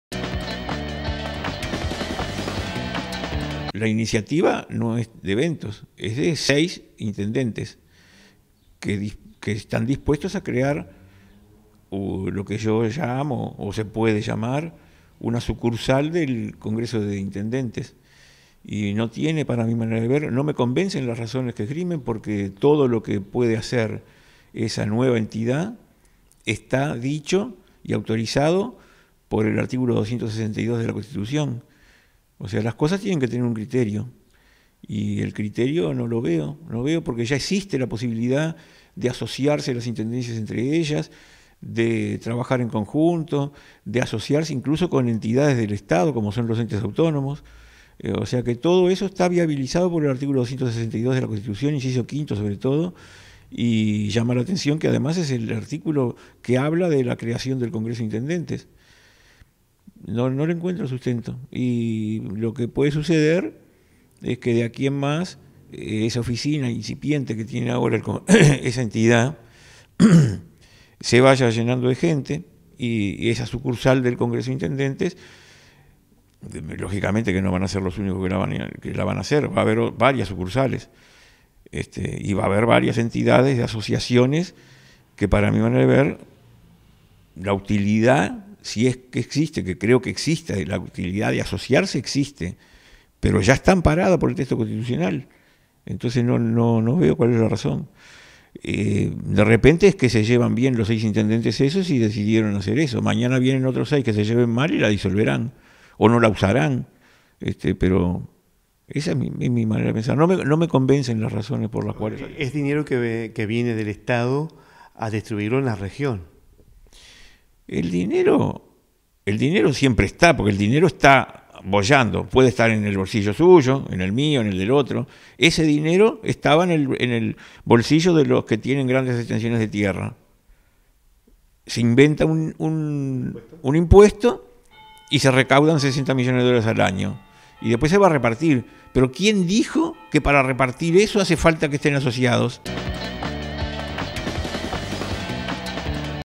Regional Norte Congreso Intendentes: Opinión de Ramón Appratto
El edil del Partido Nacional Ramón Appratto votó negativamente la integración de Paysandú a la Regional Norte del Congreso de Intendentes en principio porque no entiende  los motivos de la asociación, ya que la Constitución prevé los mecanismos para que las intendencias reciban el dinero del gobierno nacional, y lo mismo sucede con el marco legal del Congreso de Intendentes. La asociación "es una sucursal del Congreso de Intendentes", dijo el edil nacionalista.